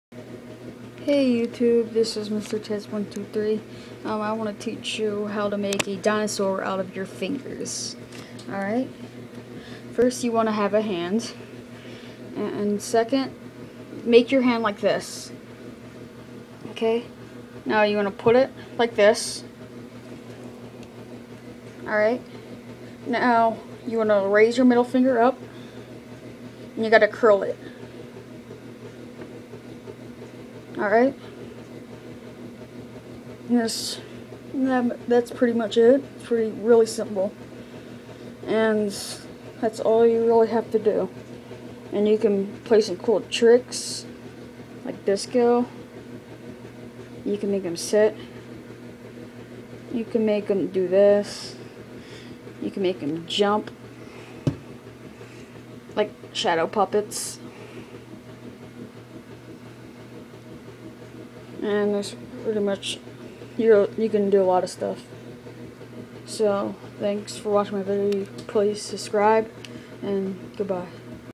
How to make a dinosaur out of your hands
It's fun make him do tricks